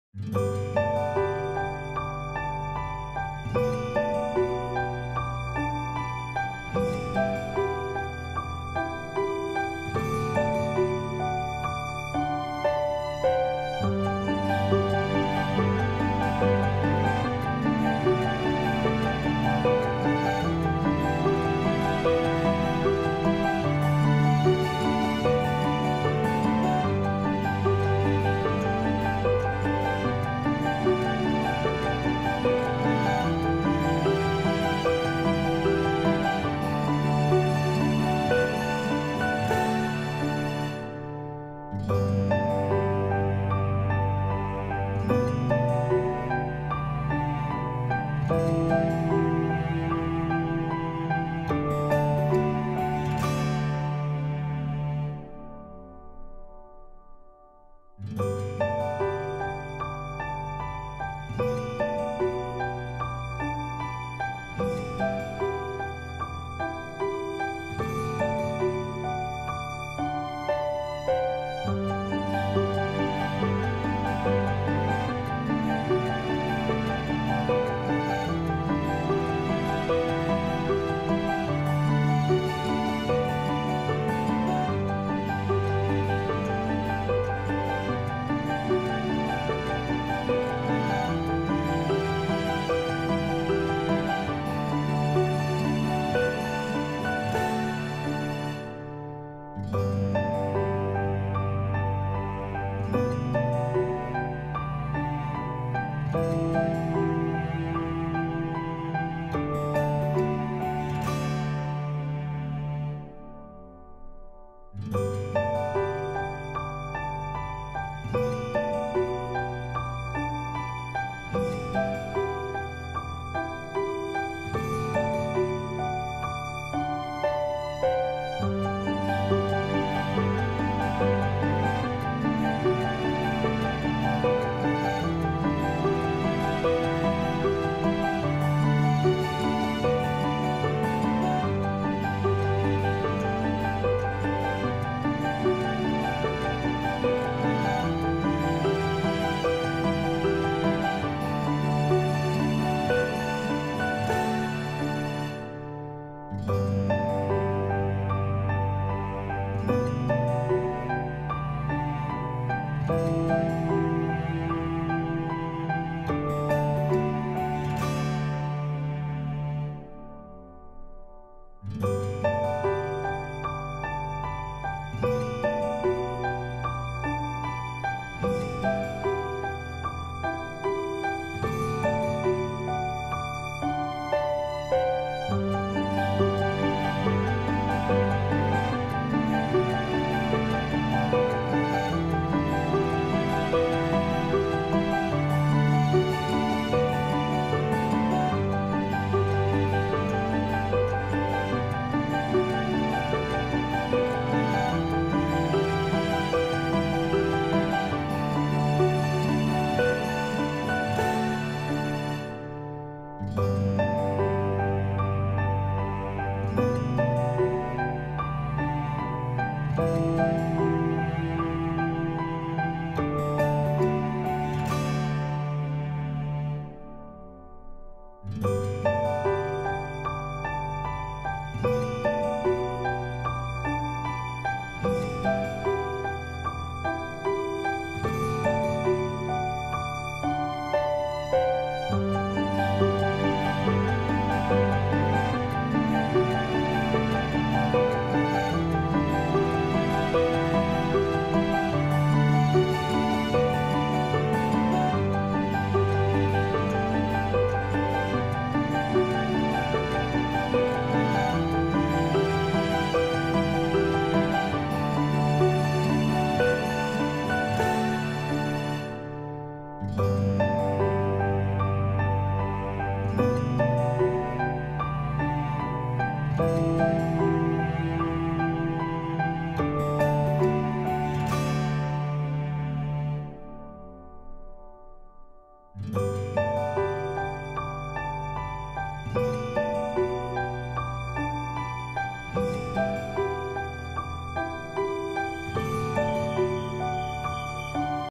Plasticcovertransformation ideas/2transformation products#sewwithme
Hi friends
This video i will show how to transform plastic covers into useful item